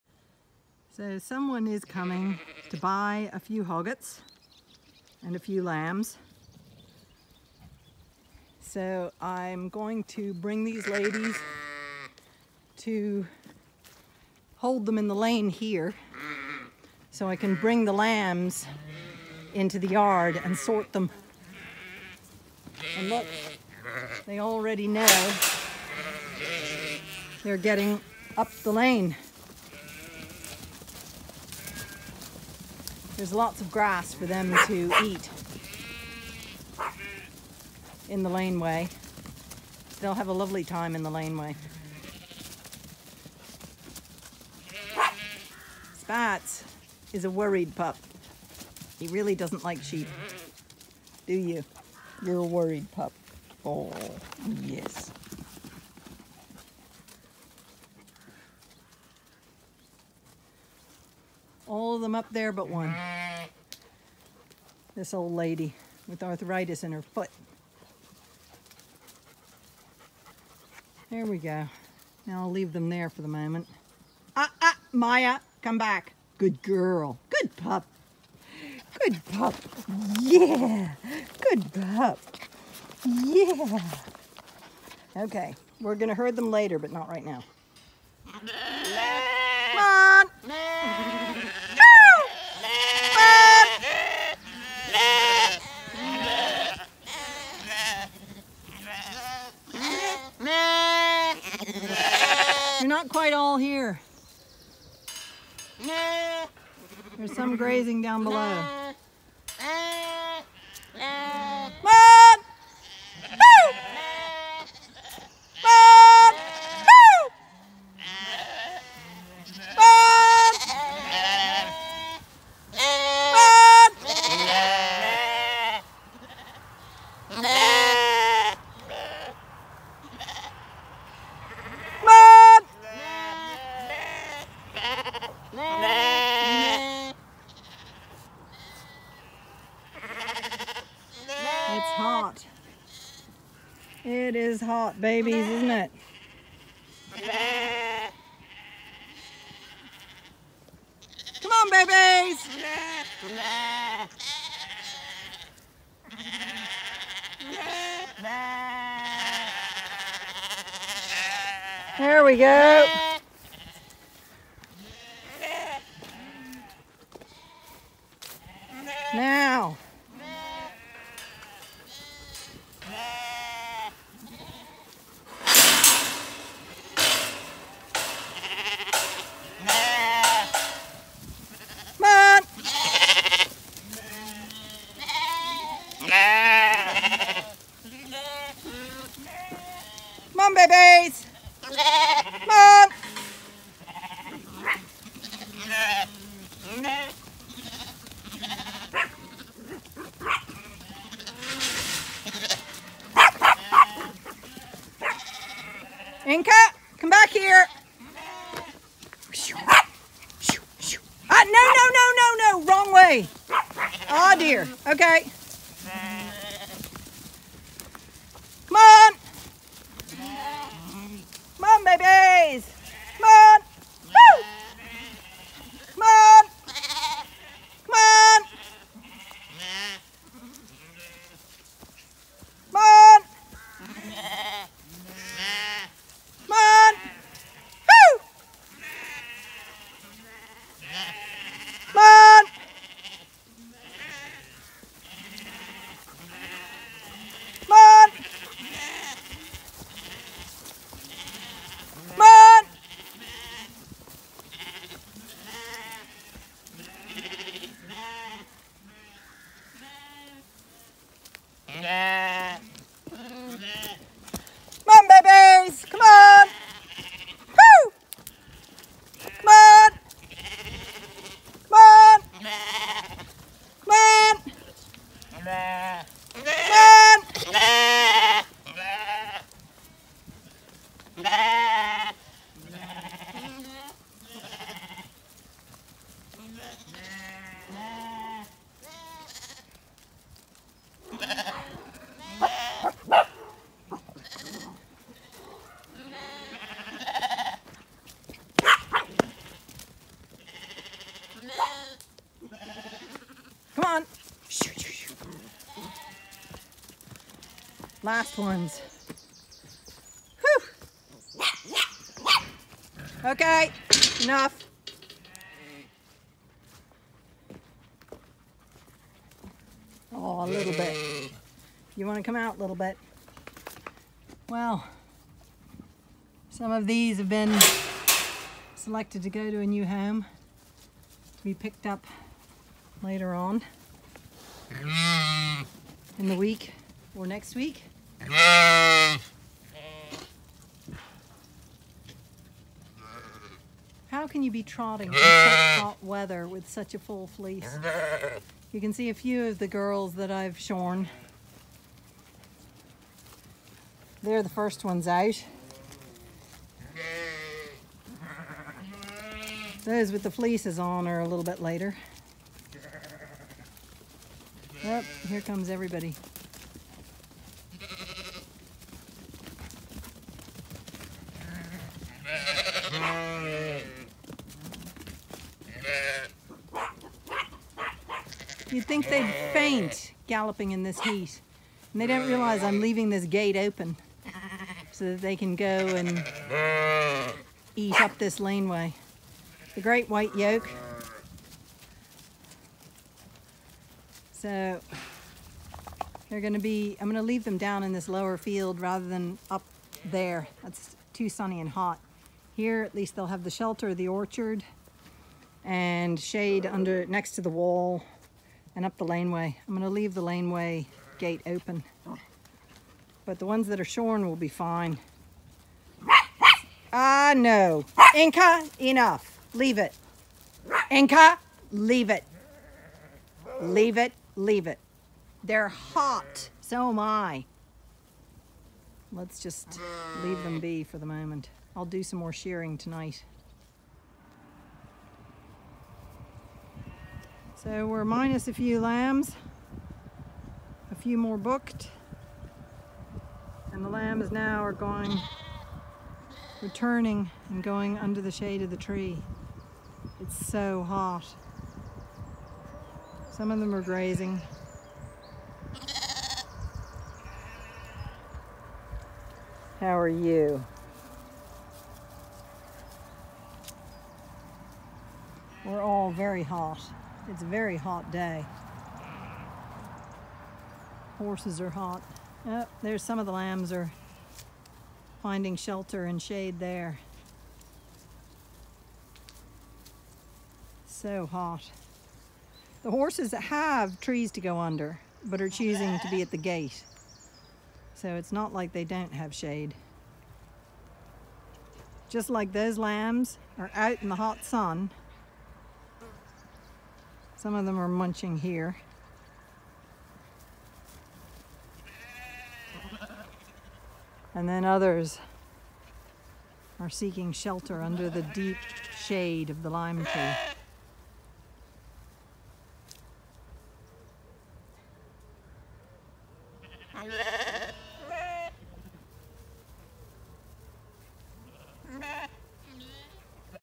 It takes some Calling in this melting heat to get lambs for a buyer’s viewing
A meltingly hot day to bring in ewes & lambs. After selections have been made loaded up and departed, a few ewes booked for pick up later next week. Remaining Lambs in a smaller flock head into shade of trees to escape direct heat.